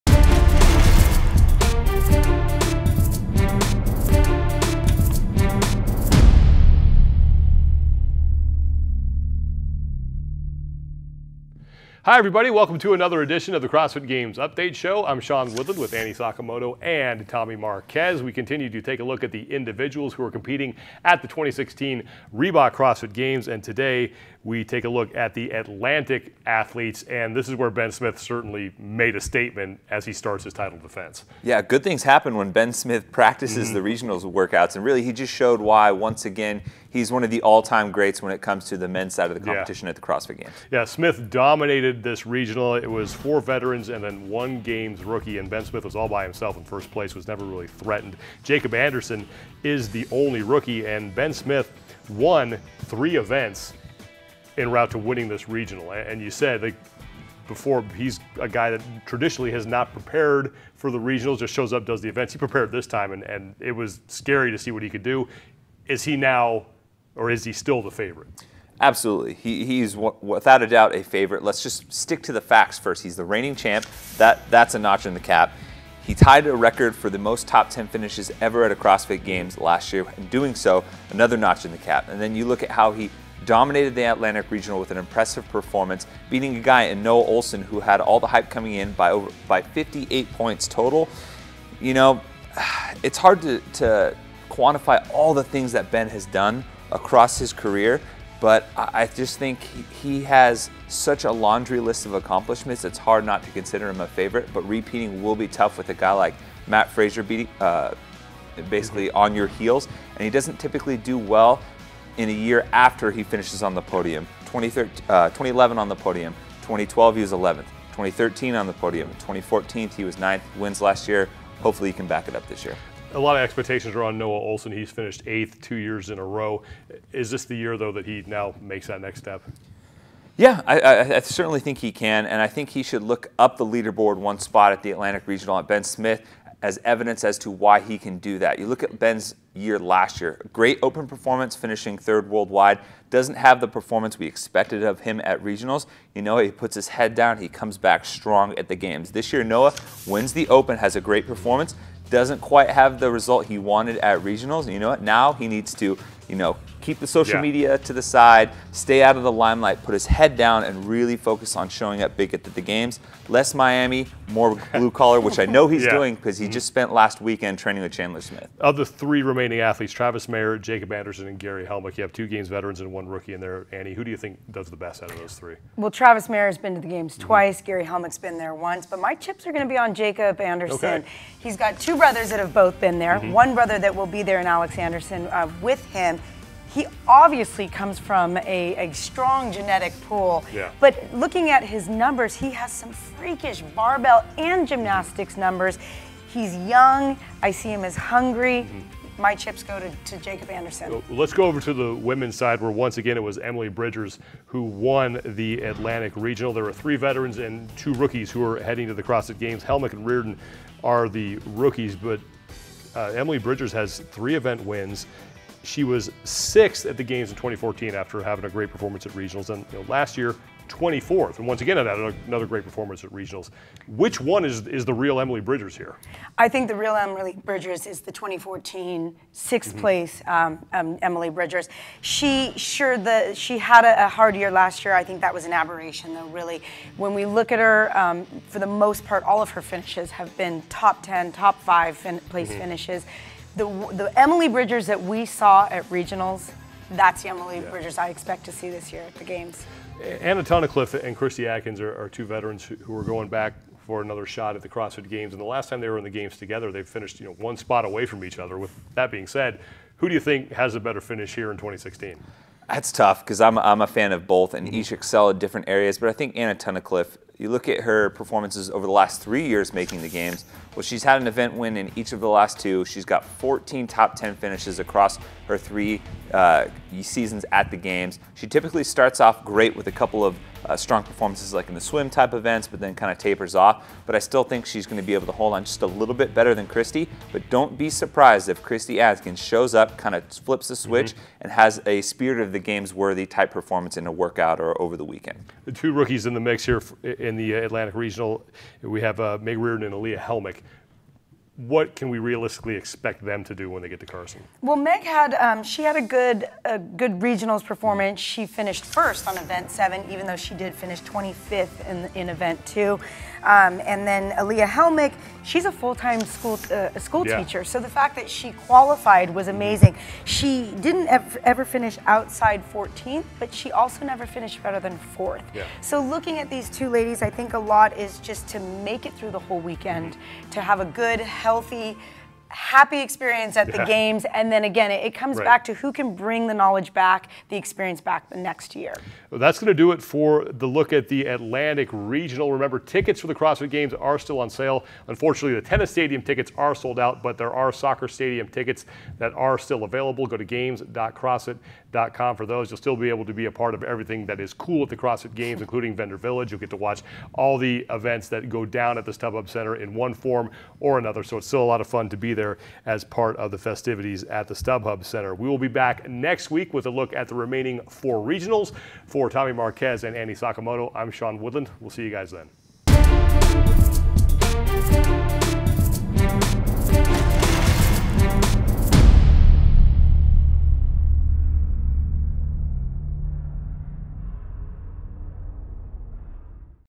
Update Show: Atlantic Qualifiers
CrossFit Games Update Show host Sean Woodland, Games analyst Tommy Marquez and guest host Annie Sakamoto take a look at the top five men and women headed to Carson out of the Atlantic Region. They discuss 2015 champion Ben Smith's chances at a repeat year, as well as Noah Ohlsen's shot at the podium. On the women's side, Games veterans Emily Bridgers and Anna Tunnicliffe go head to head, while rookies Alea Helmick and Meg Reardon attempt to make a name for themselves at their first trip to the Games.